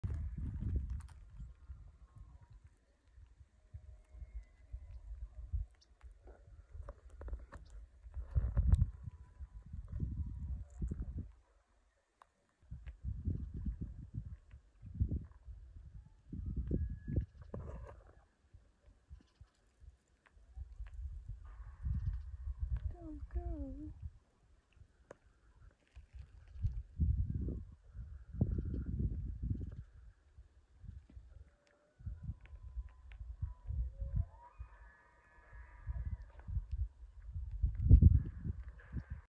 Annual trip to see the foliage and listen to the elk.
Every year on the first few weeks of October we head up to the Panguitch Lake area to enjoy the fall foliage of Cedar Canyon and listen to the Elk bugle. Its become a treasured tradition.